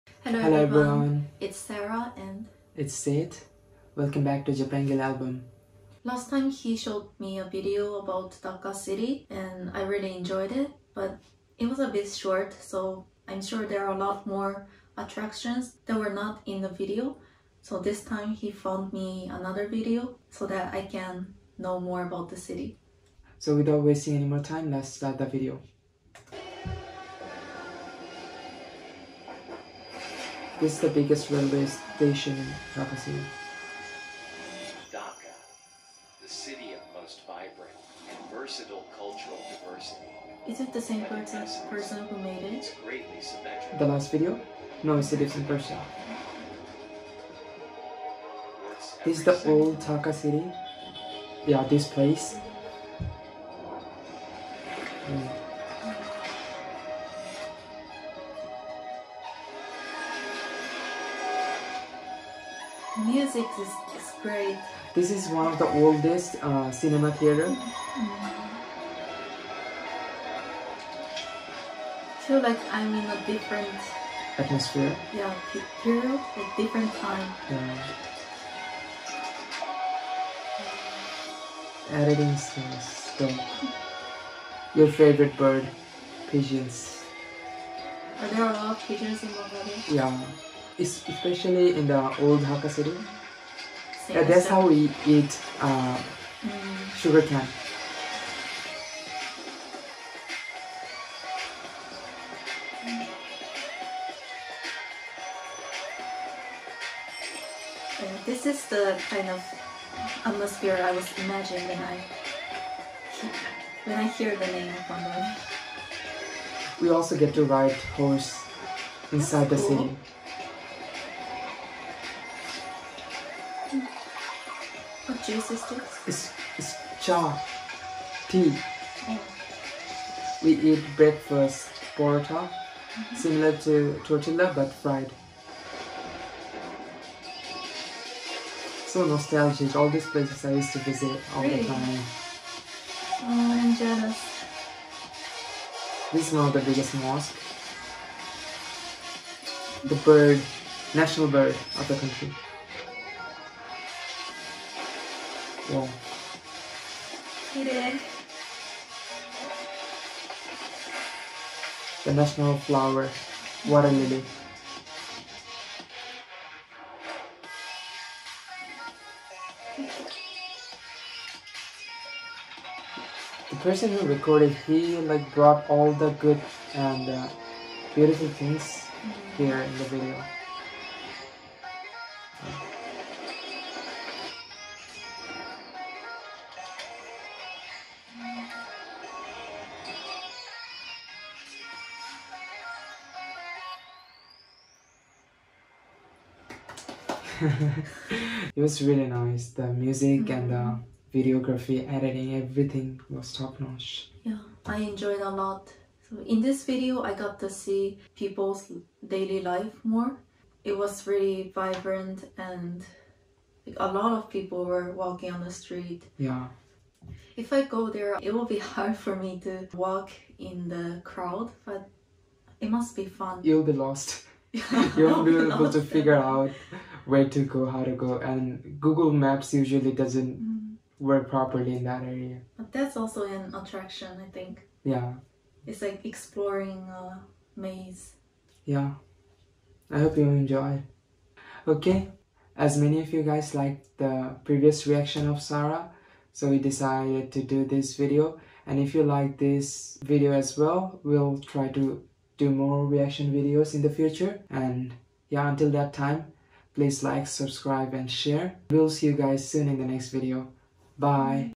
Japanese Girlfriend Reacting to Dhaka, Bangladesh part 2 // Taste of Dhaka Reaction
Hello everyone, welcome to Japangali Album.
It's Sid (Bangladeshi boyfriend) and Sara (Chinese x Japanese girlfriend).

In today's video, Sara reacted to the video titled Taste of Dhaka by Prince Hasan